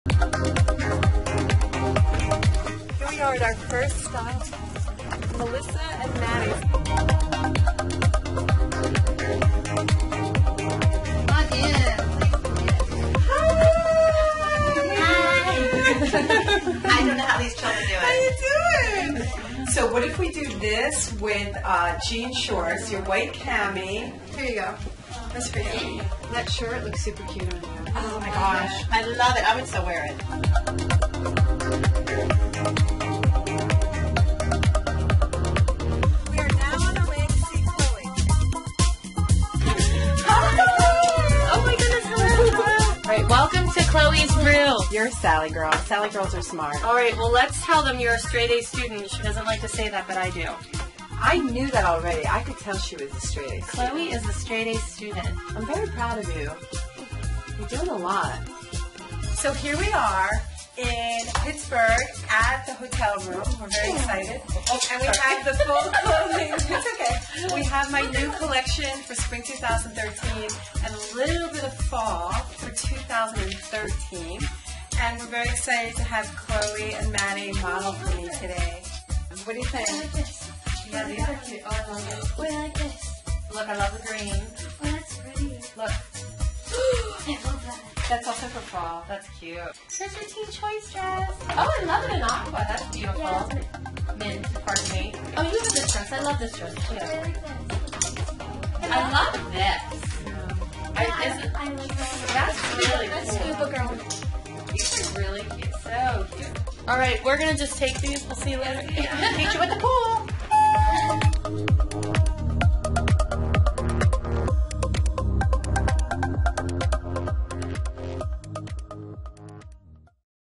Chloe & Maddie - Behind the S.M Photoshoot scene
THIS VIDEO IS OWNED BY SALLY MILLER!
Chloe and Maddie are 10 years old dancer.
Sally Miller Photoshoot (behind the scenes)